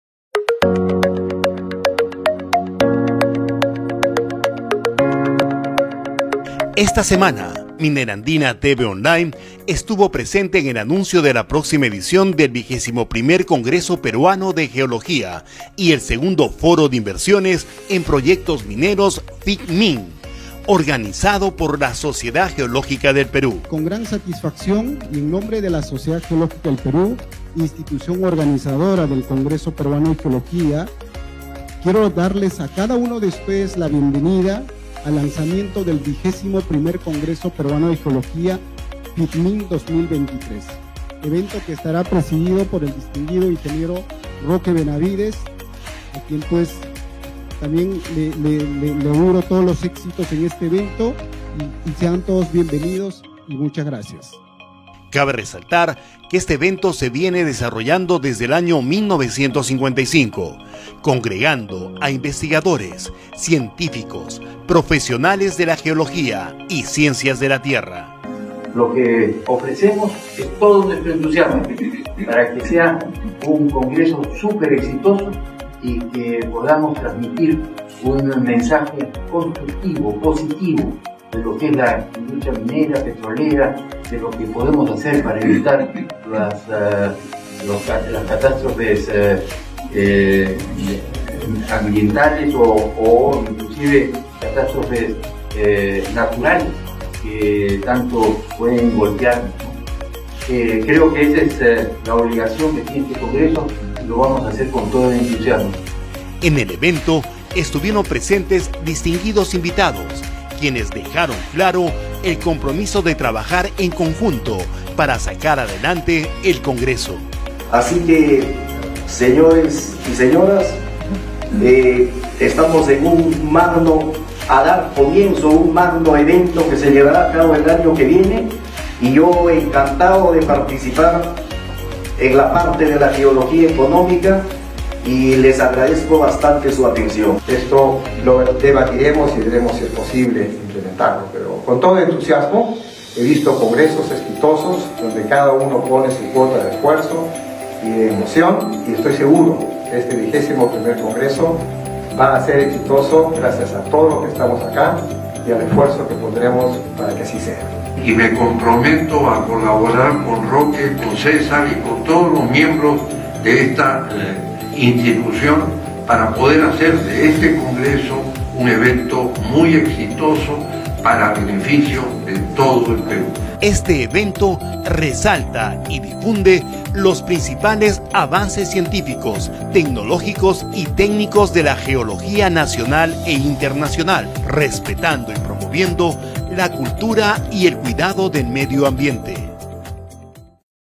Lanzamiento del XXI Congreso Peruano de Geología y FIPMIN
Lanzamiento del XXI Congreso Peruano de Geología y II Foro de Fomento de Inversiones en Proyectos Mineros (FIPMIN), bajo la presidencia del Ing. Roque Benavides. Se realizará del 23 al 27 de octubre de 2023 en la Universidad Nacional Mayor de San Marcos . "Más investigación, mejor productividad".